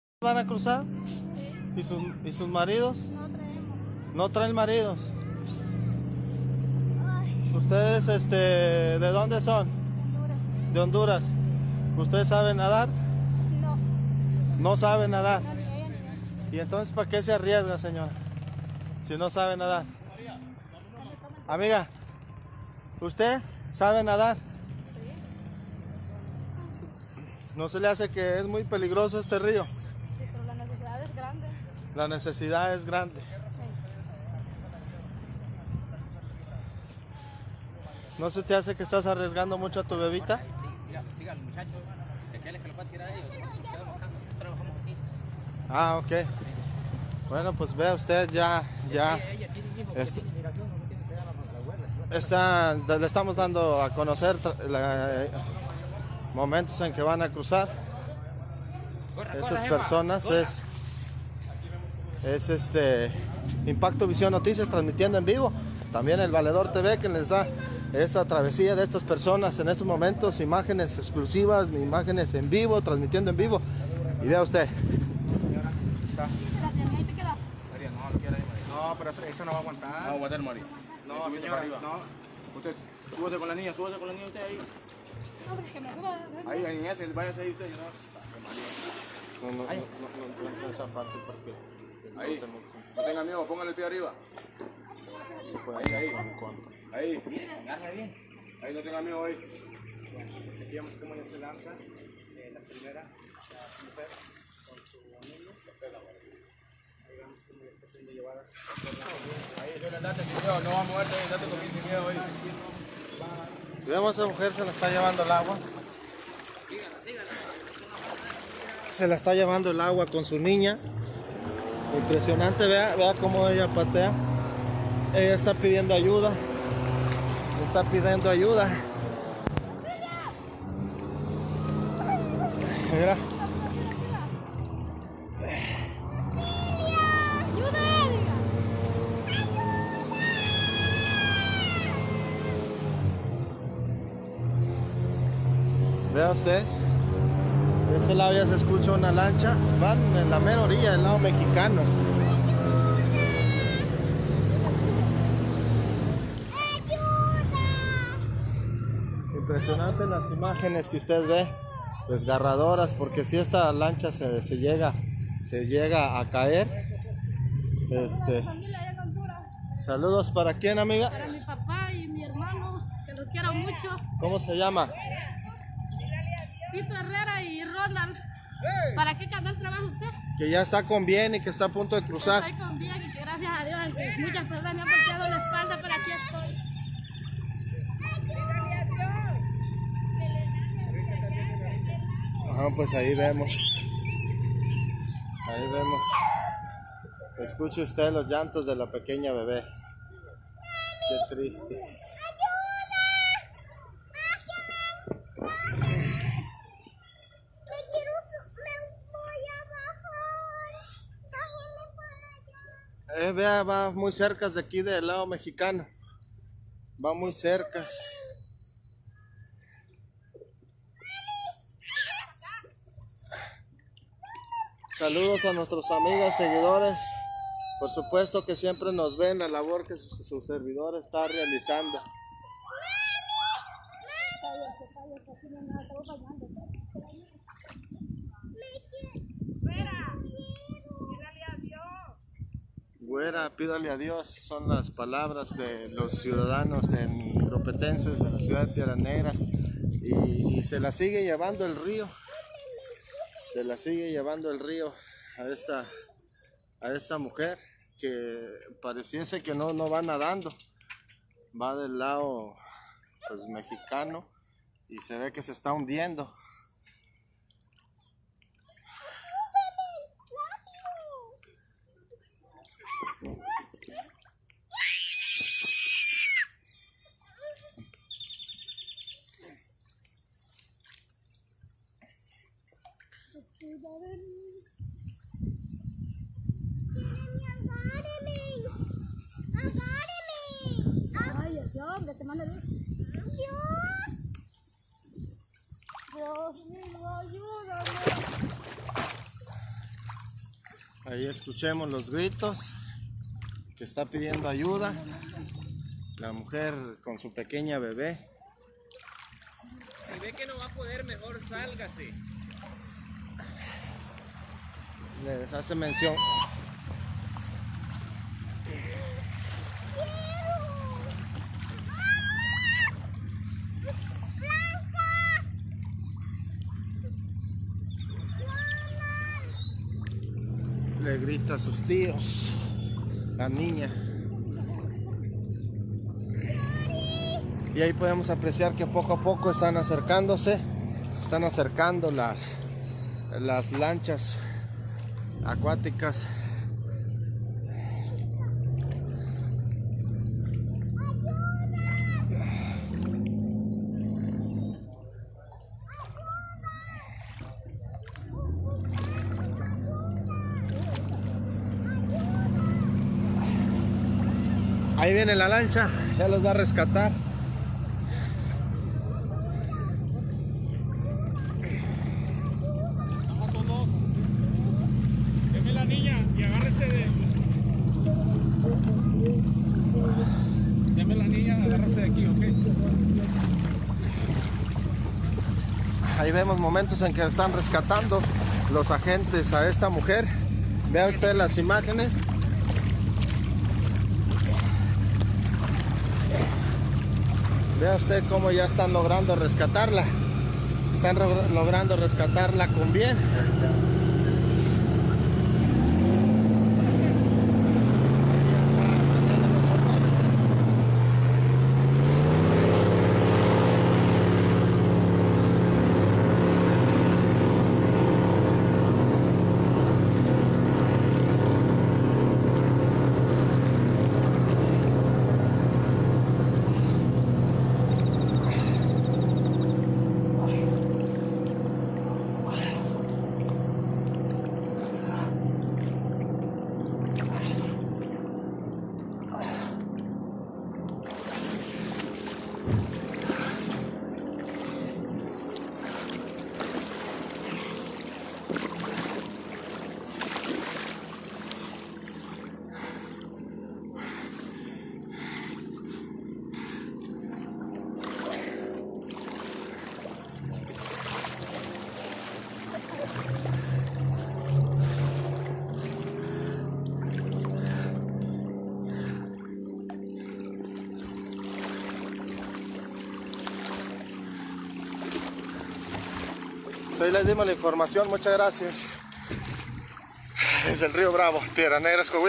Caravana Madre Migrante De Honduras Cruzan Rio Bravo Y Llegan A USA Agentes Migracion Ayudan
la caravana madre migrante de honduras llegan a USA ayudados por los agentes de migracion de USA quienes los rescatan y envian a un albergue shelter llamado las carpas en texas, ahi los alimentan 3 veces al dia y los ayudan. la caravana madre migrante de honduras han logrado entrar a USA y conmover el corazon de Donald Trump ya que les da asilo